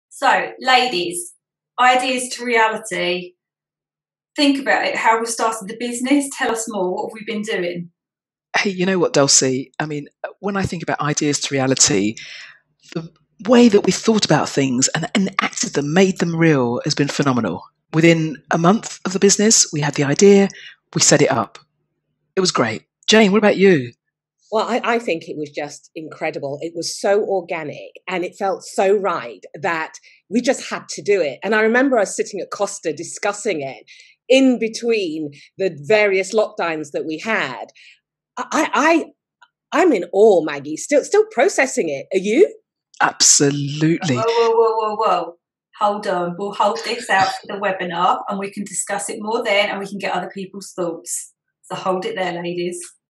From Ideas to Reality
A preview of what the I-Cubed team will be discussing in their first webinar, taking place on 18 February at 6pm.

Register to join our webinar here: @